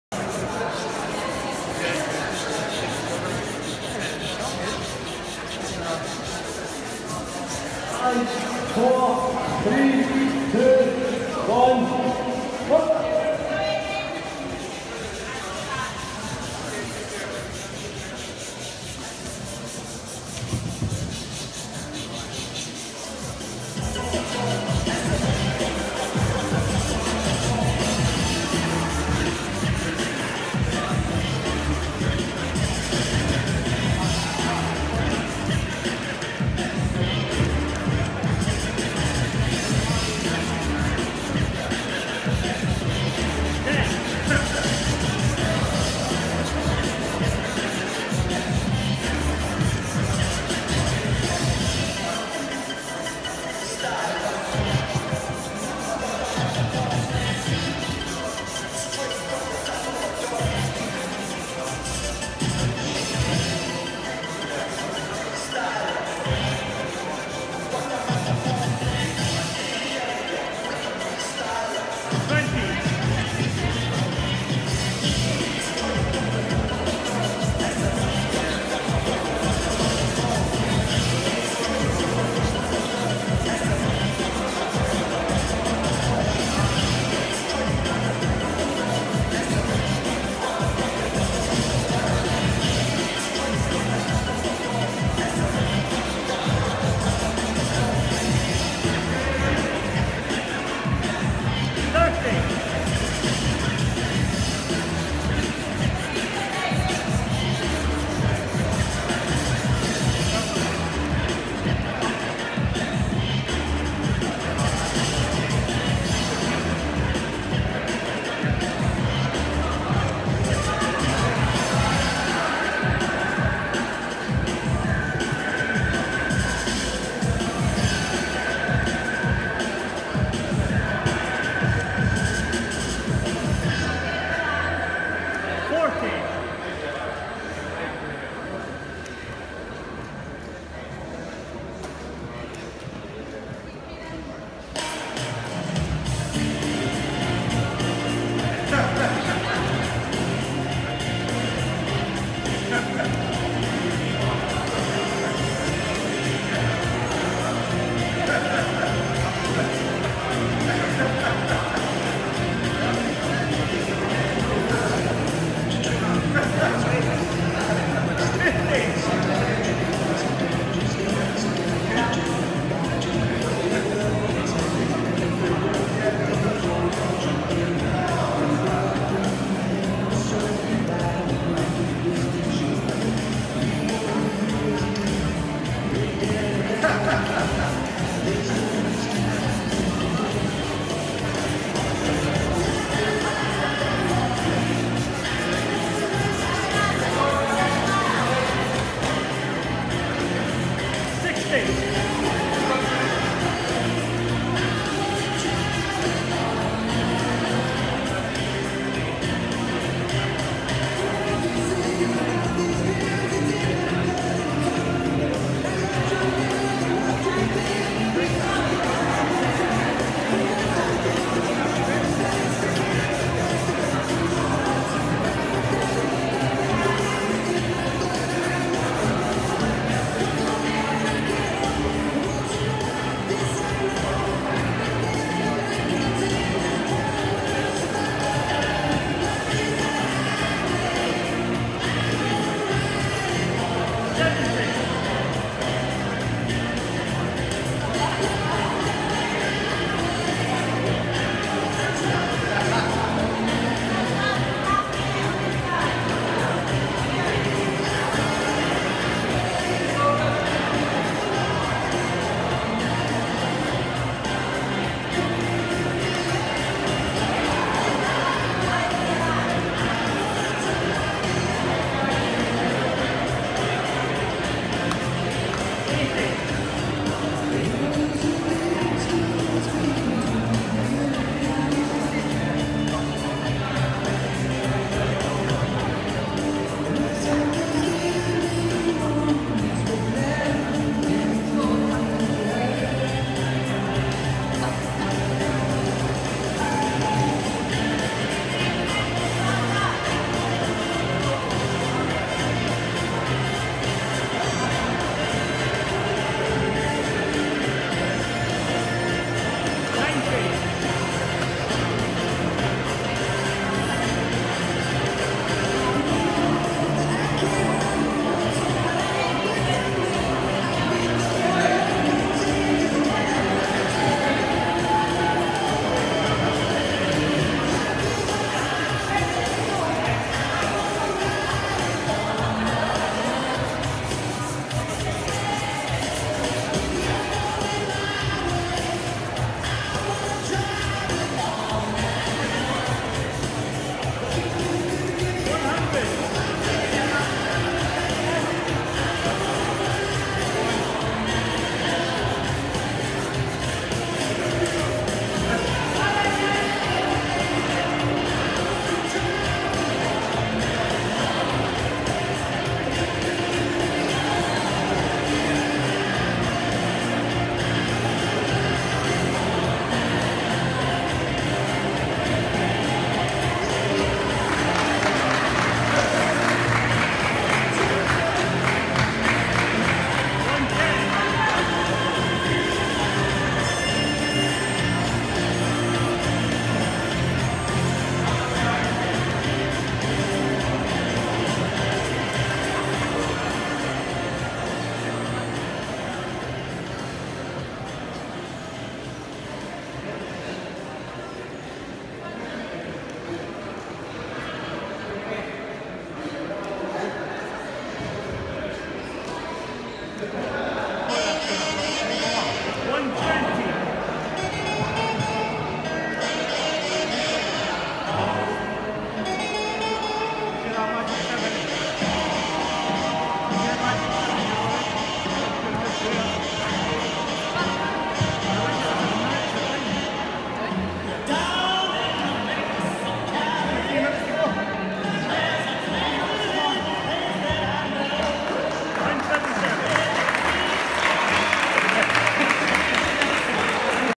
Seonagh @Scottish championships 12/8/17
8kg biathlon snatch 127 rep